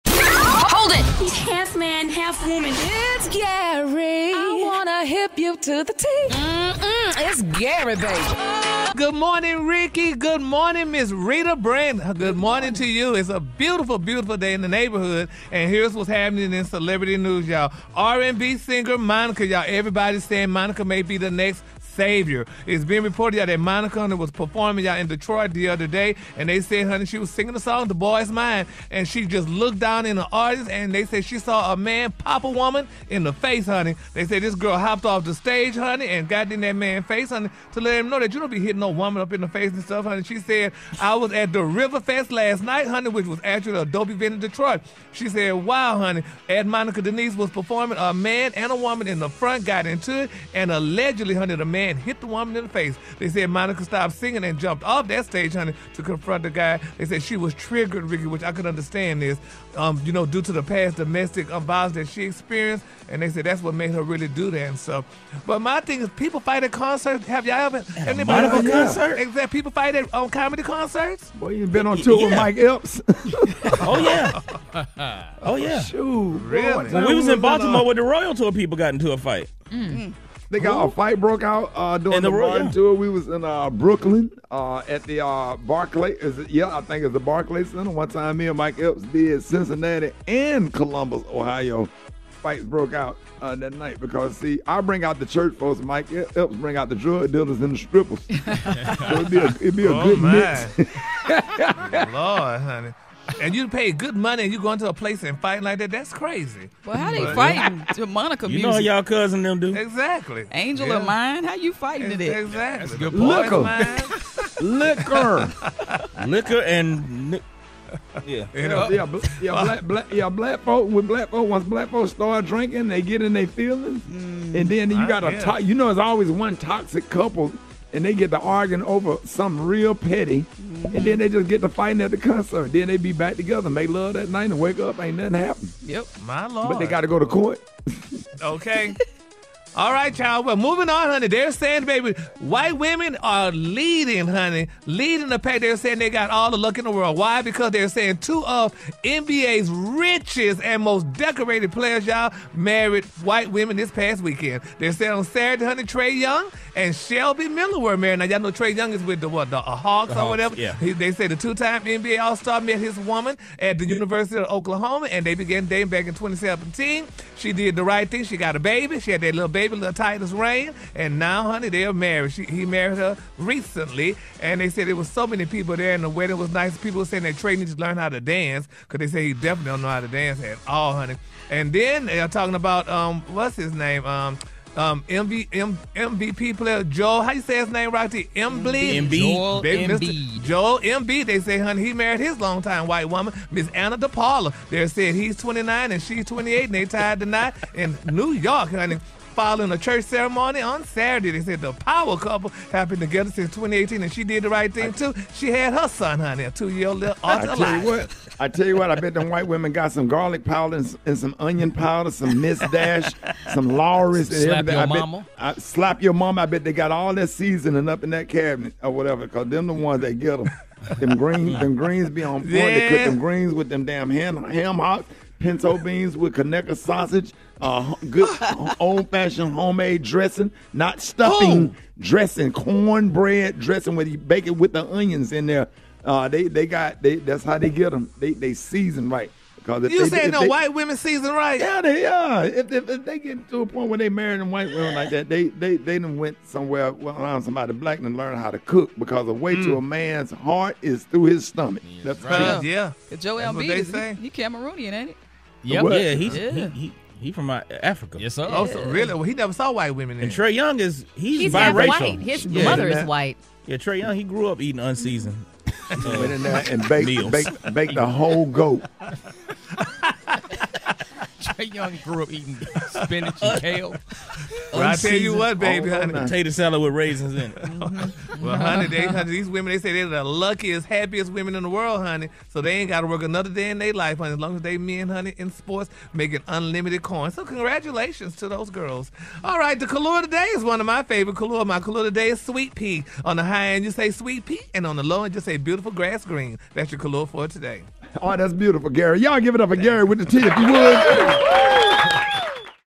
Monica Confronts A Man | White Women Winning
Listen to this Tea about Monica, Trae Young, Joel Embiid, and two White women who are winning... GaryWithDa Tea.com